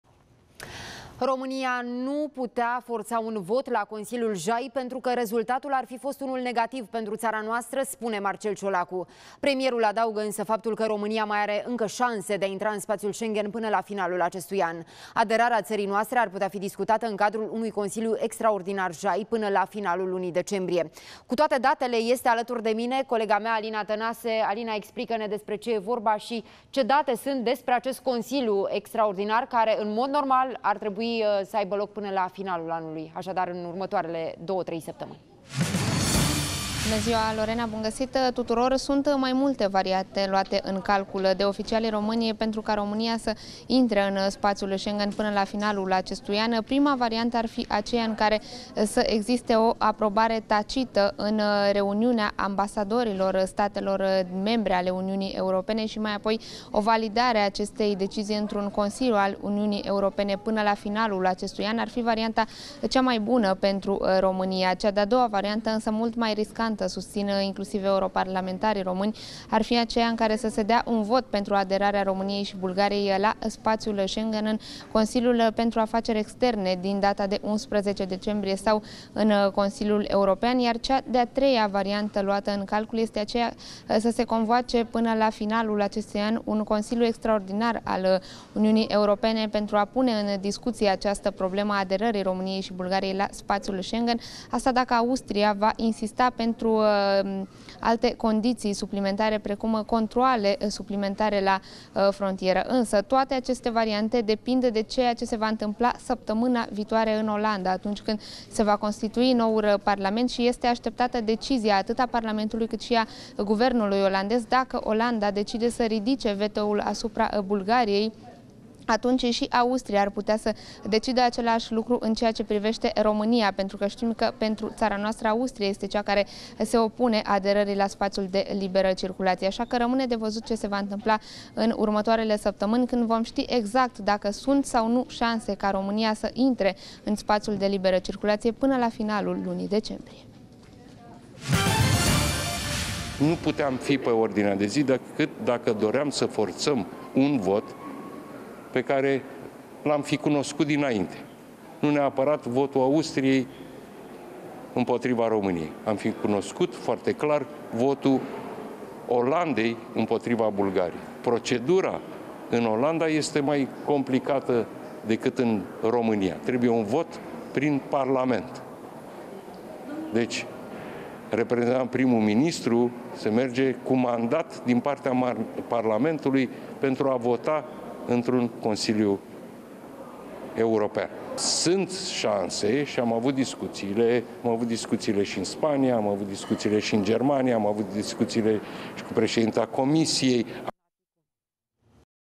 Consiliul JAI s-a terminat fără un vot pentru intrarea României în Schengen
Consiliul JAI s-a încheiat fără un vot pentru intrarea României în Schengen, însă premierul Marcel Ciolacu susține că țara noastră încă mai are șanse pentru a primi un răspun favorabil până la finalul acestui an.